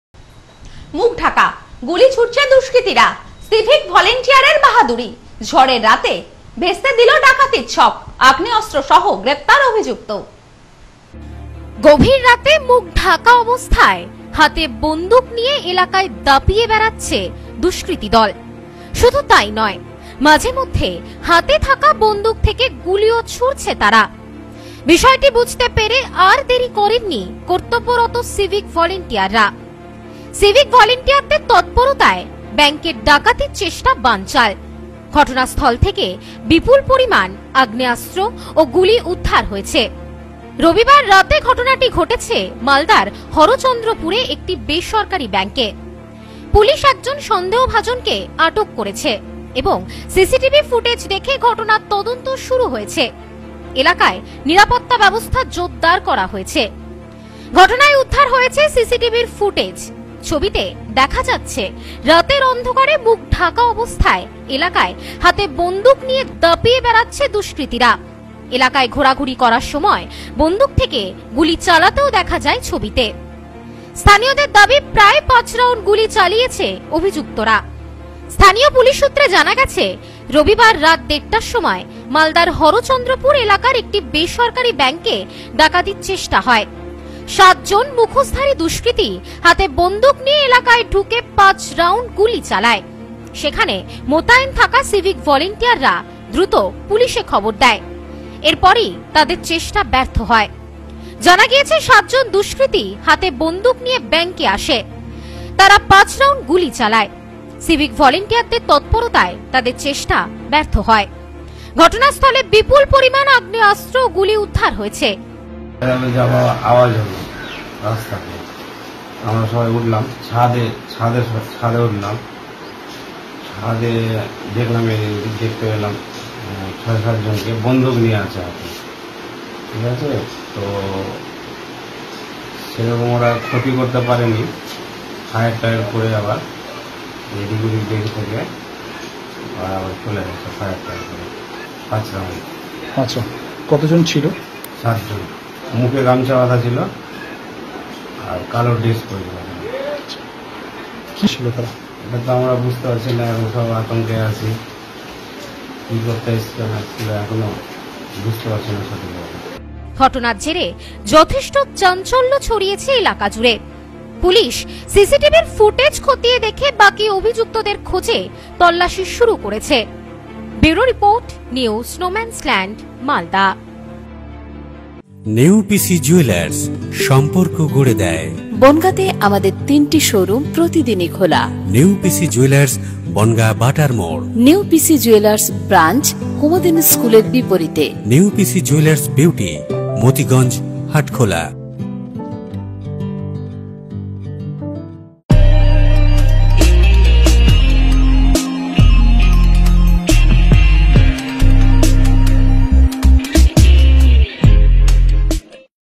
মুখ ঢাকা, গুলি ছুড়ছে দুষ্কৃতীরা! সিভিক ভলেন্টিয়ার এর "বাহাদুরি"
মুখ ঢাকা, গুলি ছুড়ছে দুষ্কৃতীরা! সিভিক ভলেন্টিয়ার এর "বাহাদুরি" ঝড়ের রাতে ভেঙে গেল ডাকাতির ছক। আগ্নেয়াস্ত্র সহ গ্রেপ্তার অভিযুক্ত।